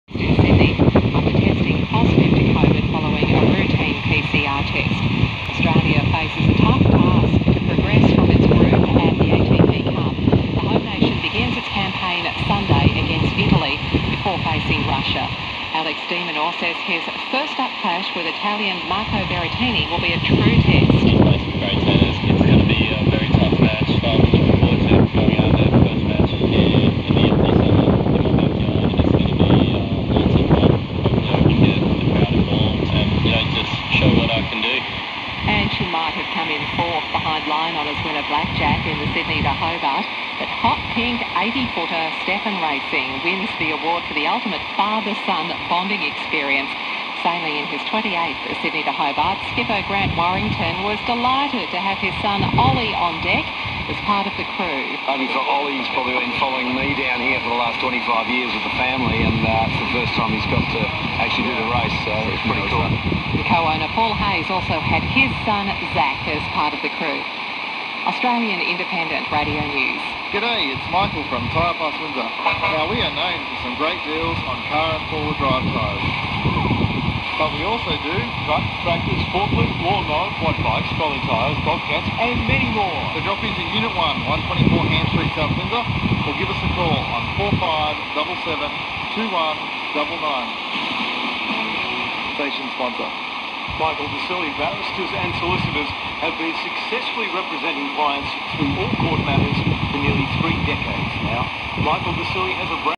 Hawkesbury Radio TCBL 89.9 Received in Bowral NSW
One of the seven radio stations vying for a community radio license in the Richmond/Windsor area. Caused a lot of fights between owners of those stations. Had I turned in hours earlier, I would be picking up Starter FM.